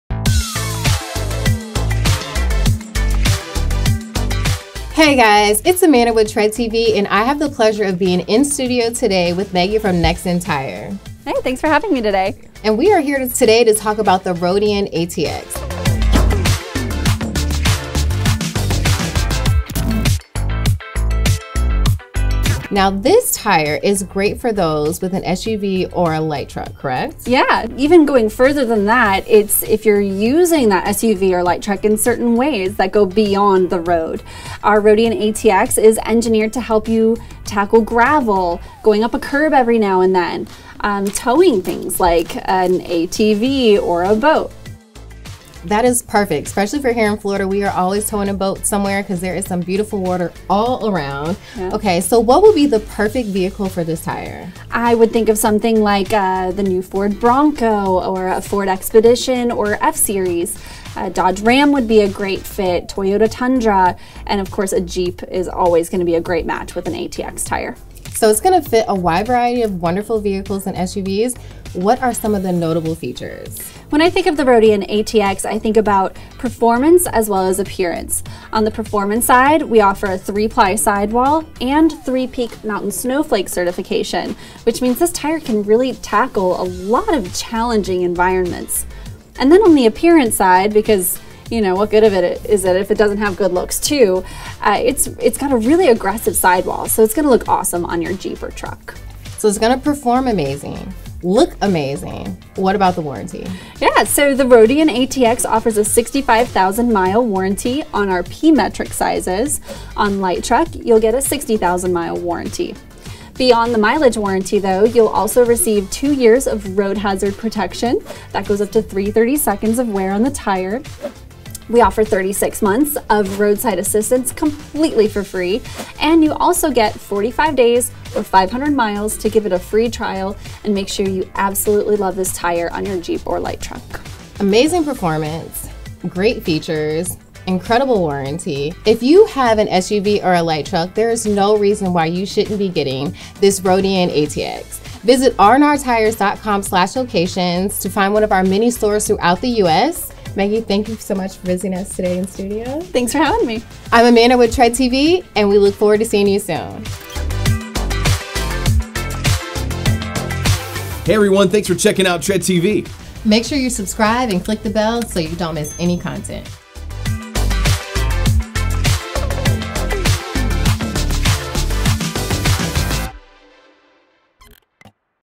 Nexen Roadian ATX OFFICIAL Review | RNR Tire Express
This week on TreadTV, we're joined by Meggie with Nexen to discuss the Roadian ATX Tire! We discuss the ins and outs of the ATX, the warranty and the types of vehicle this tire is best suited for. Check it out and if you want to get a set for yourself head to find your nearest RNR!